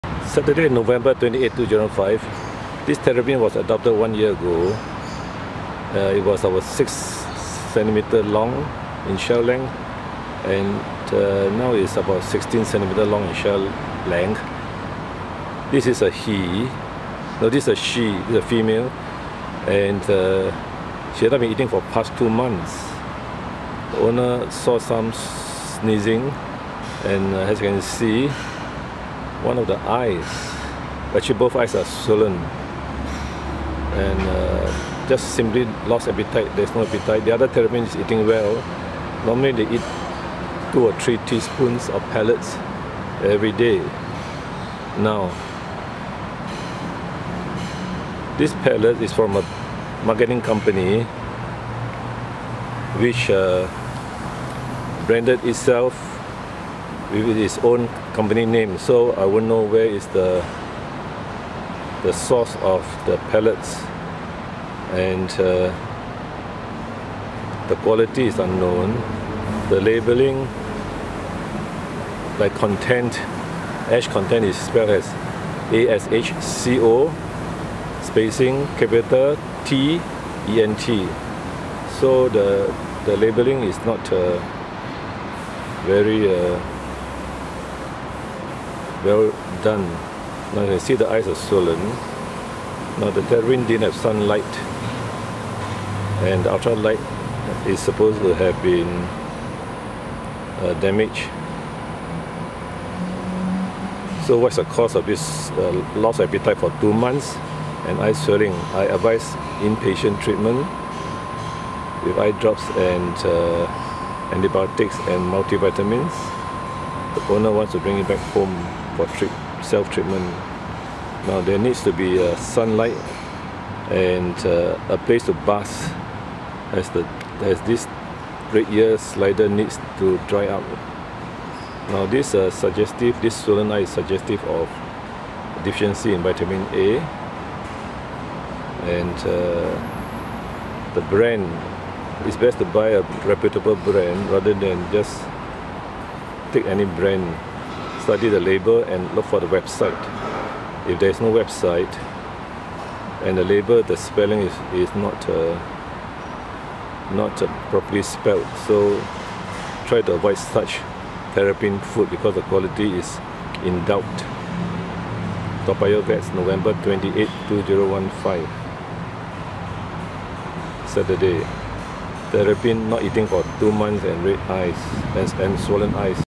A terrapin has swollen eyes and anorexia
Sat Nov 28, 2015
A female terrapin has not been eating for the past 2 months. Her mate eats well. The terrapin pellets were from a company which brands the product. The quality of the pellets is suspect.

Eyelid swelling may be due to infection or lack of Vitamin A.  A reputable brand of terrapin food, sunlight, medication and management is advised.